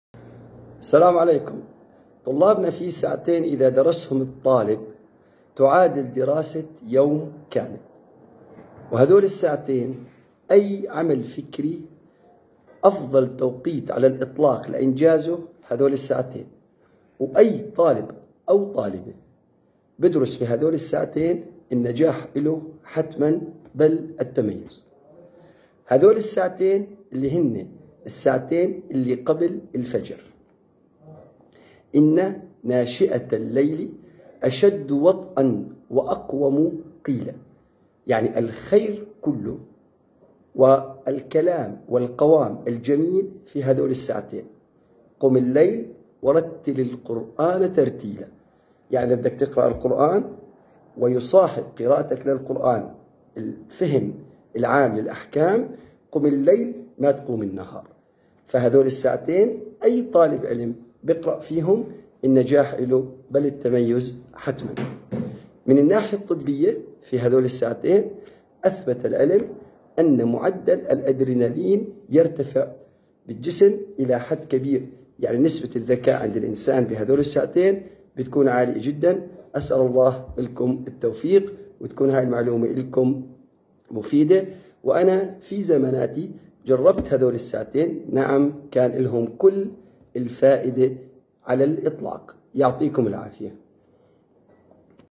نصيحة هامة مع الأستاذ عيسى بني ياسين
قال رسول الله صلى الله عليه و سلم: "اللهم بارك لأمتي في بكورها"
نصيحة هــــــــامة في الدراسة لا تفوتها 🔥🔥😍
مع الأستاذ عيسى بني ياسين 😍👌🏻💯